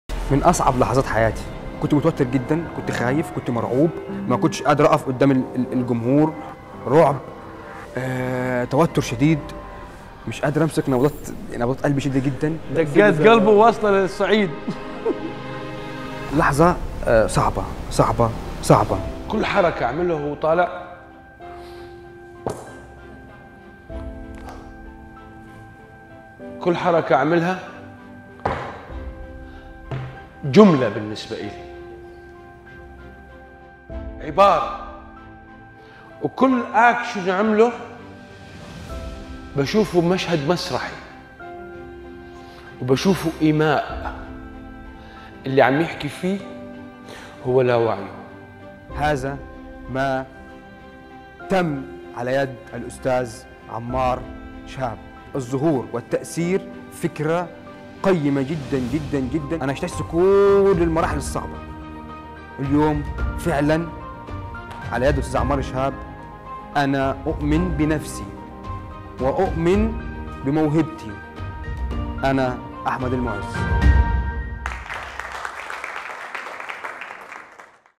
تجربة أحمد حافظ في ورشة الظهور والتأثير مع الاستاذ عمار شهاب
من تجربة المتدرب أحمد حافظ في ورشة الظهور والتأثير لشهر يونيو 2022